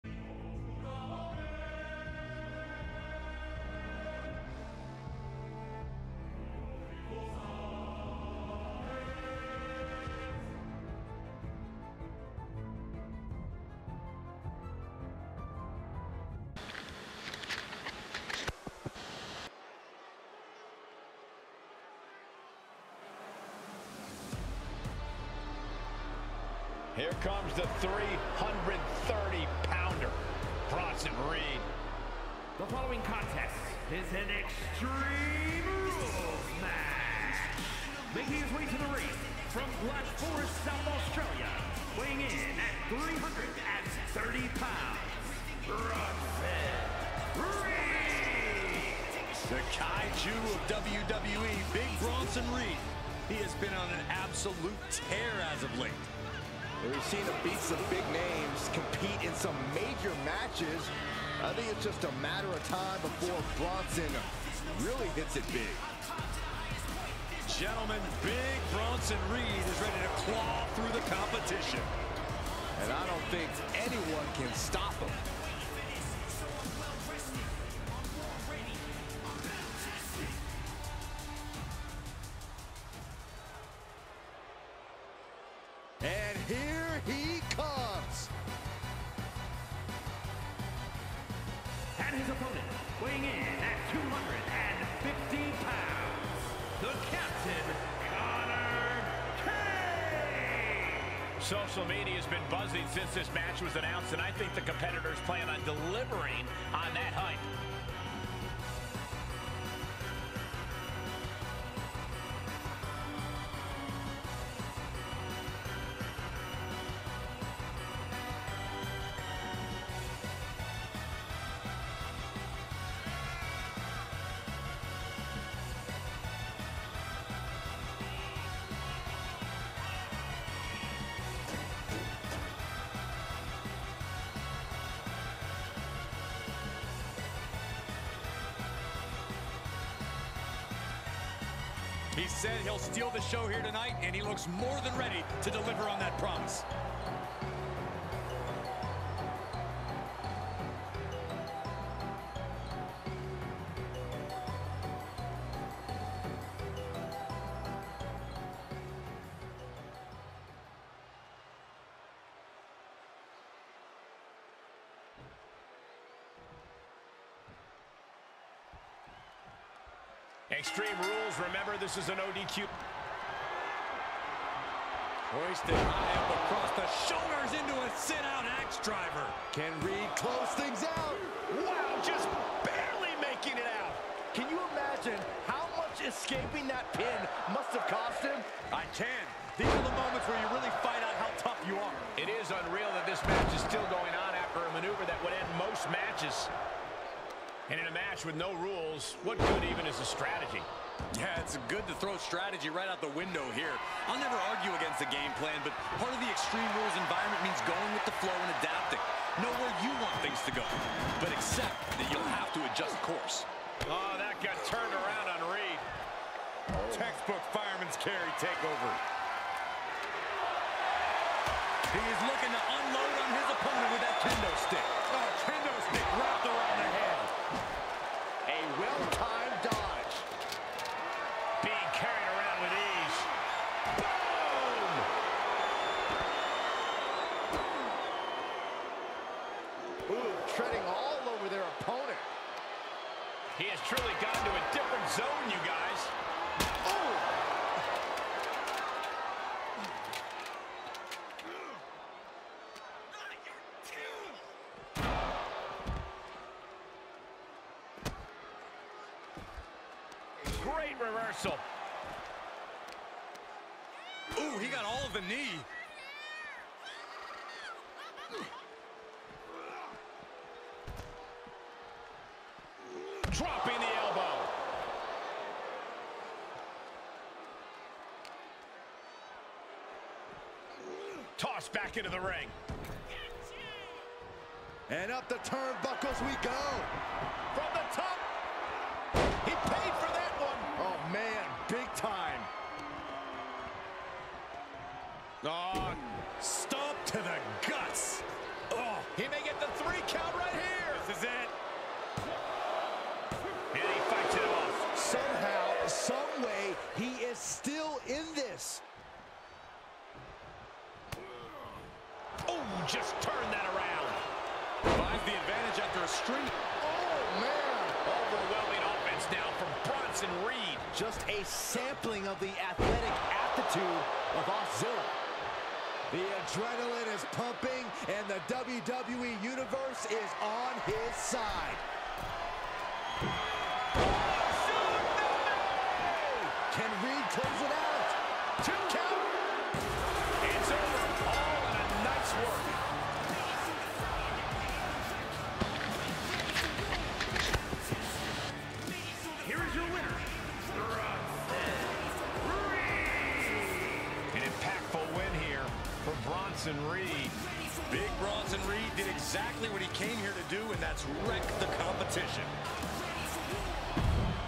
Triple Crown Tournament Round 4: Bronson Reed vs Koffin-K!
This will be a round robin type tournament so sit back and enjoy the show!